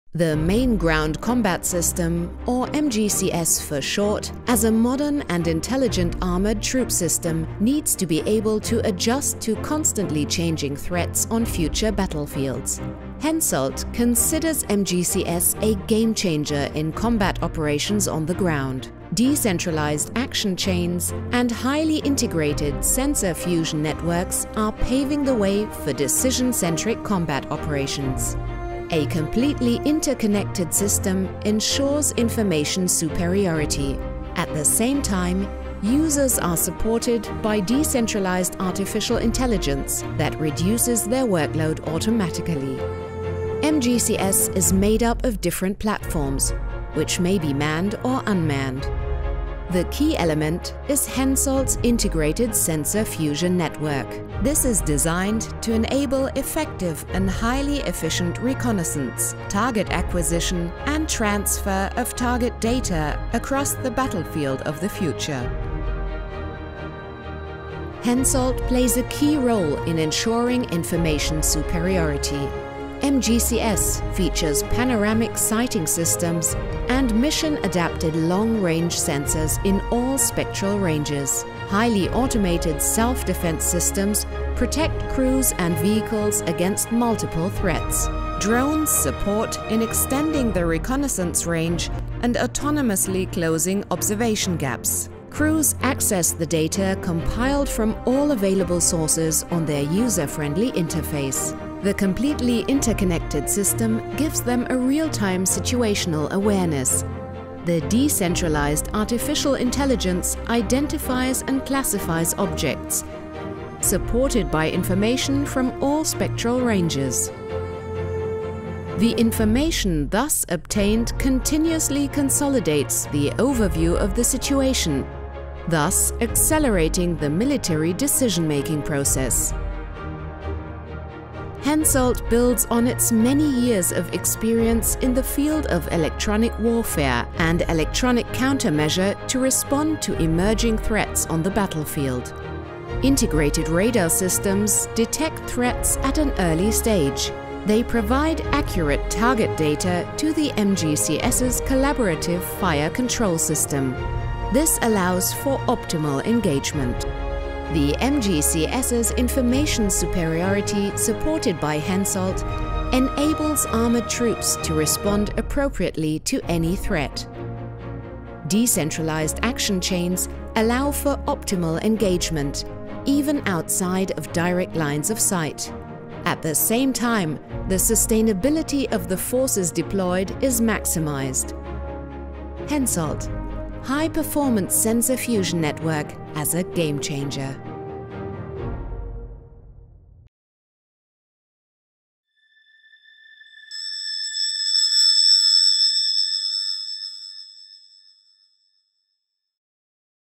HENSOLDT´s vision for MGCS – Game changer for armoured vehicles
Together, France and Germany strive for a new generation of armoured vehicles, the Main Ground Combat System (MGCS).

In this context, we at HENSOLDT know  that a smart network of sensors will be a real game changer for ground-based combat. As proven expert and partner for optronics, radars, avionics and spectrum dominance, we developed our vision for MGCS.

For us, the biggest opportunity for the German-French project lies in the user-oriented analysis of heterogeneous information generated by different sensors within one single system. With such a smart network supported by artificial intelligence (AI), we deliver automated and detailed battlefield data, which allows a  real-time reaction.

For us at HENSOLDT, this vision is the key technology for a decision-based combat management. Because whoever gains an informational advantage by the intelligent fusion of data also gains decision-making superiority. And at last, faster and more efficient decisions lead to dominance on the battlefield.